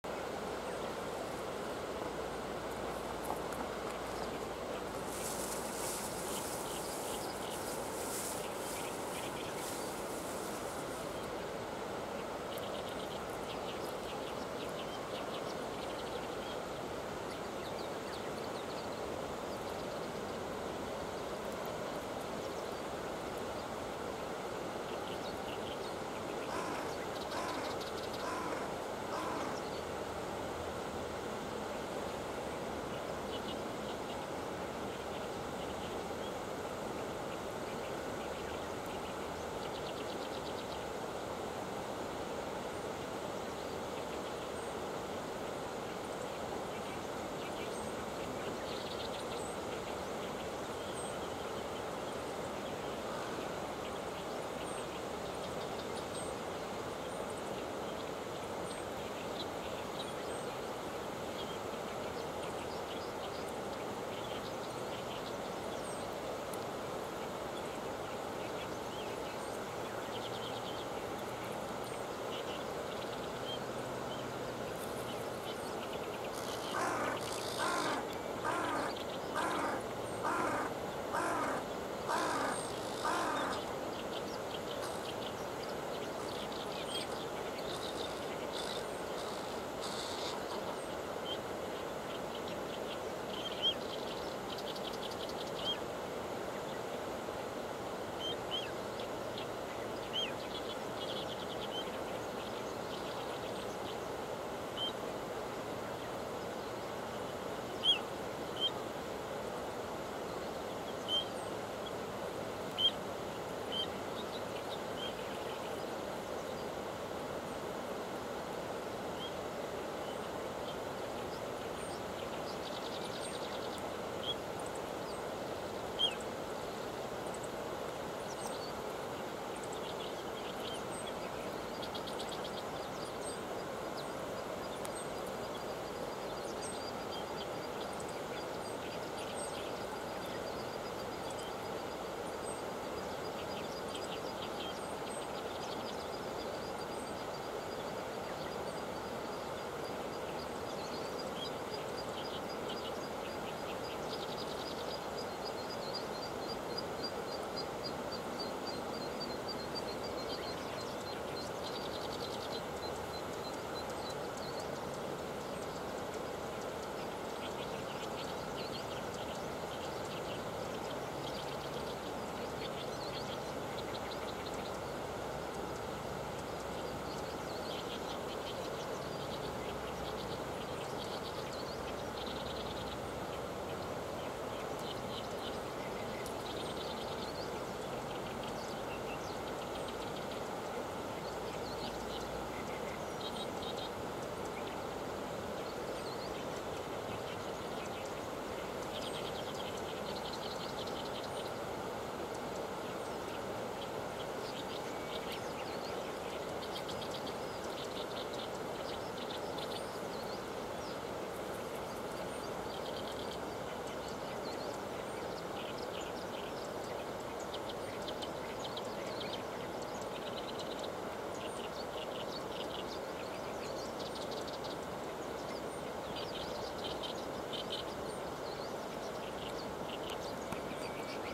K0300 大分川 モンファミリー（ハシボソガラス）Oita River MON Family (Wild Carrion Crow)
6月20日　7：16　アオイに食パンと豆腐を与える。レイが止まり木から水辺へ下りてくる。アオイが口移しでレイに餌を与える。モンは食パンと豆腐を銜えて帰る。

K0300　大分川　モンファミリー（ハシボソガラス）แม่น้ำโออิตะ ครอบครัว MON (ซากศพอีกา)
20 มิถุนายน 7:16 ให้ขนมปังกับเต้าหู้ AOI  RAIY ลงจากคอนไปที่ริมน้ำ  AOI เลี้ยง RAIY แบบปากต่อปาก  MON หยิบขนมปังกับเต้าหู้กลับบ้าน